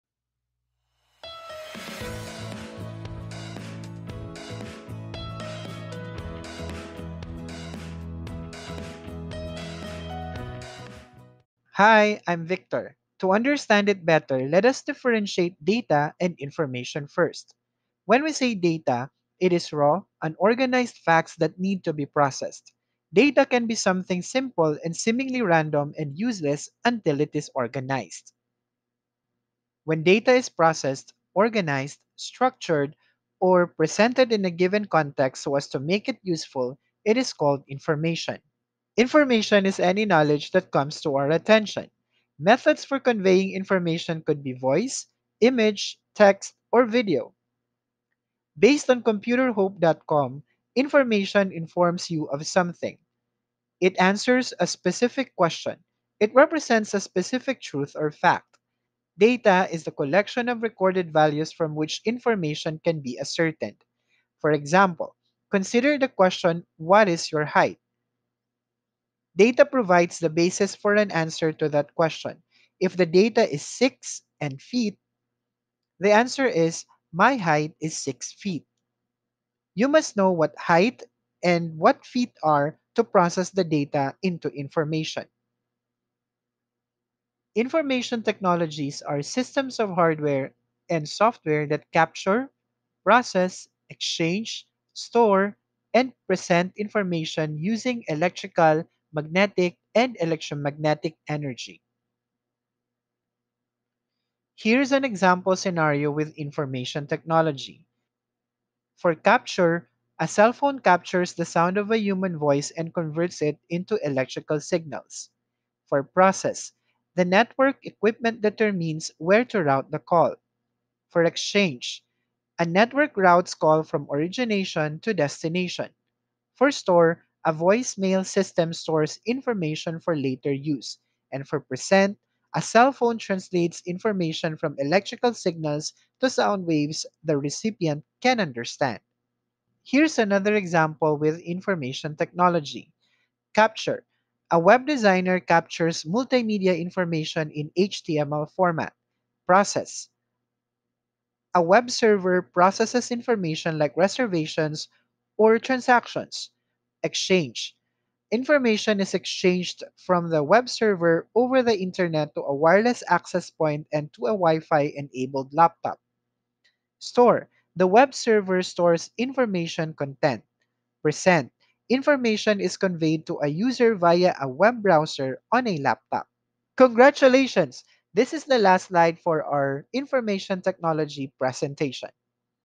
What is Information Technology? Explained Simply!
By the end of the video, students will be able to
1. Differentiate data and information;
2. Define information technology;
3. Provide a scenario in which information technology is present; and
4. Determine the composition of information technologies.

Thanks to Bitmoji's Chrome extension for the avatar.